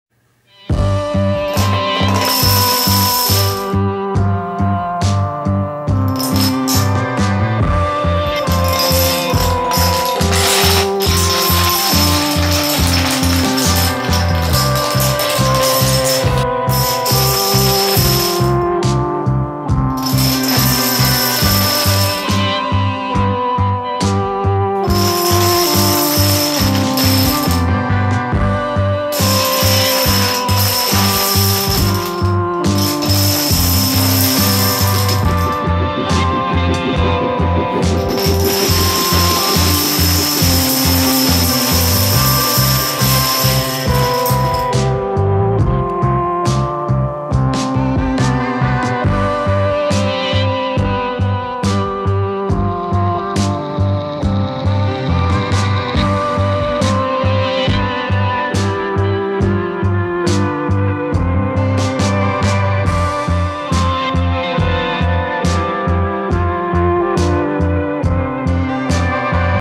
new domino technighues , tests and experiments
some of these techniques will be in my domino chain rally video